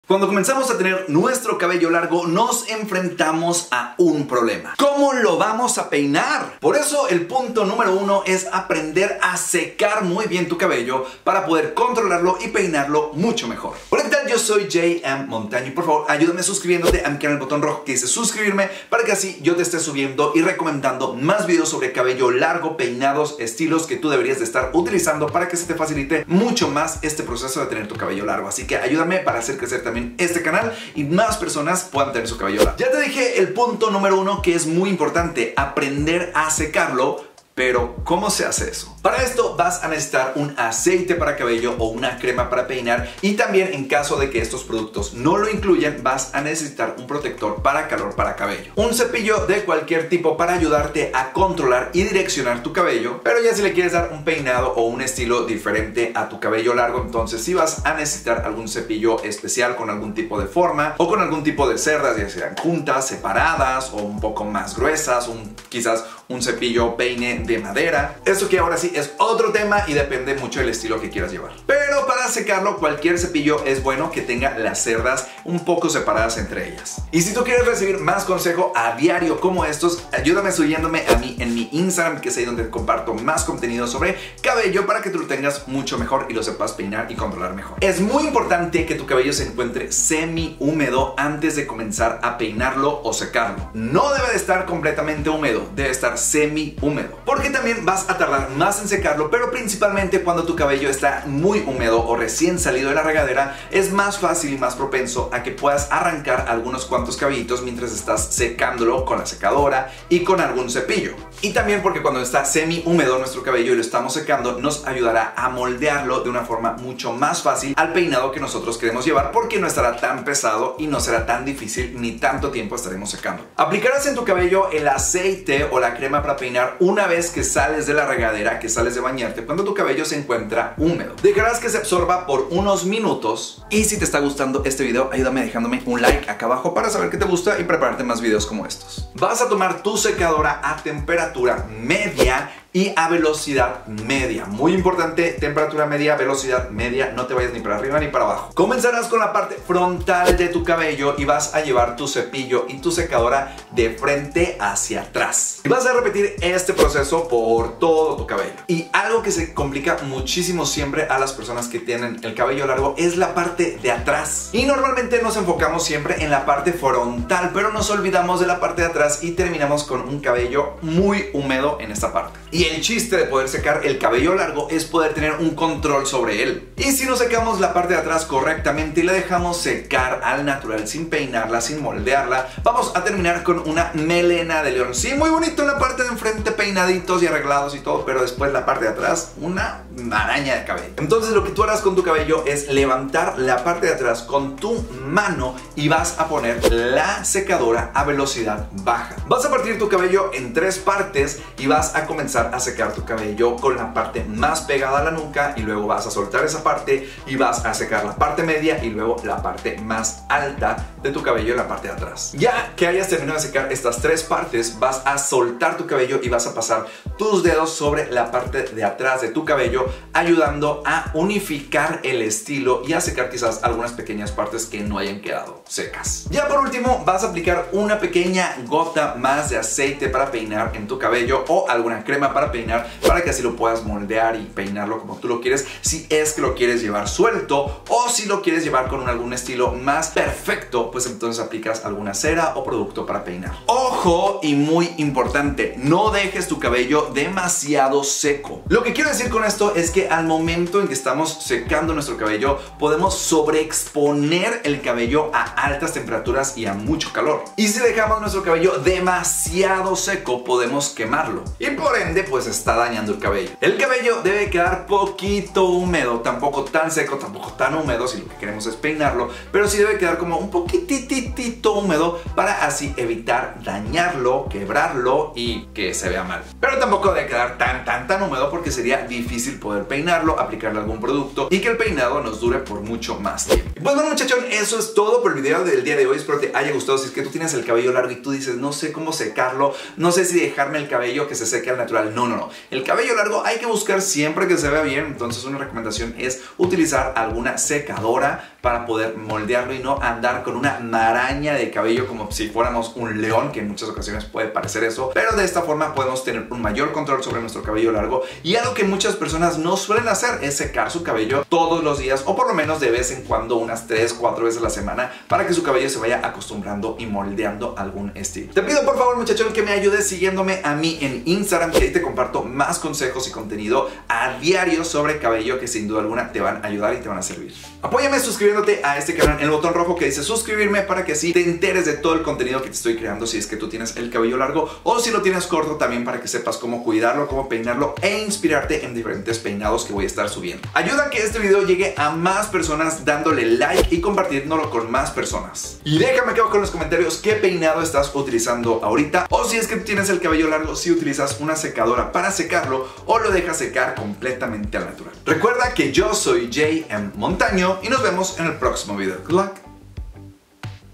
COMO SECAR CABELLO LARGO HOMBRES (Tutorial) - J.M. Montaño
Secar el cabello largo es una de las situaciones que muchos hombres nos enfrentamos y no sabemos cómo  hacerlo, o incluso lo hacemos mal y quedamos con el cabello con mucho volumen o peor de lo que estábamos.
Por eso te comparto un tutorial que te ayudará a saber como secar tu cabello largo correctamente.